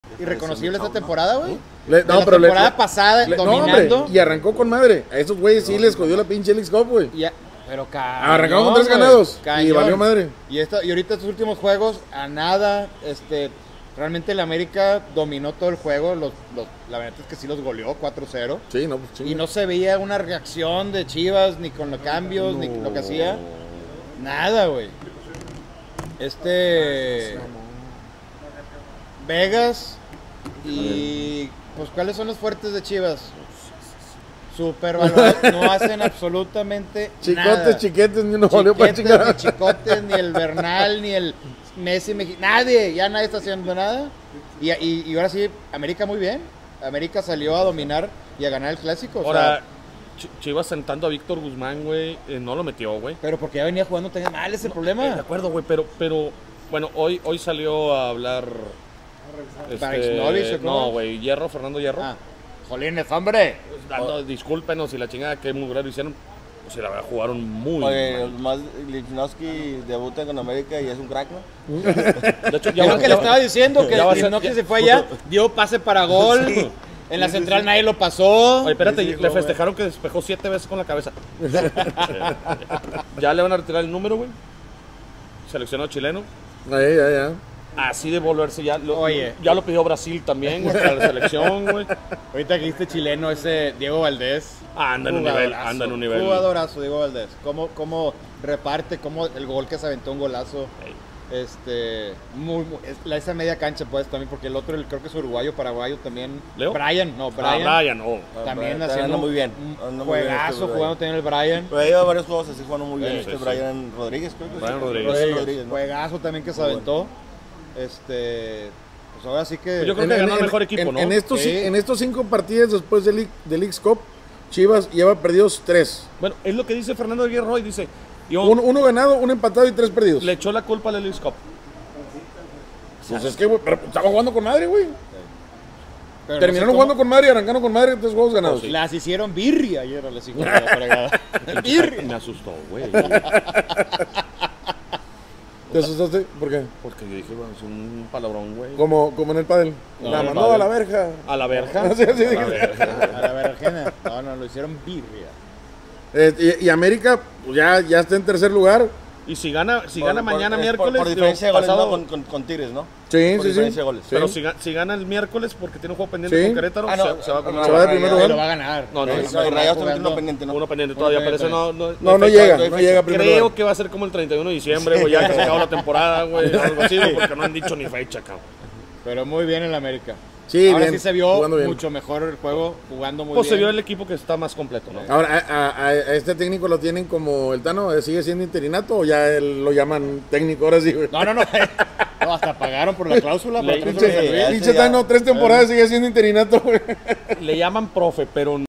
Barrida de America a Chivas, muy triste lo de Guadalajara
🦅 Barrida de America a Chivas, muy triste lo de Guadalajara y más triste el nivel de sus jugadores por ejemplo Vegas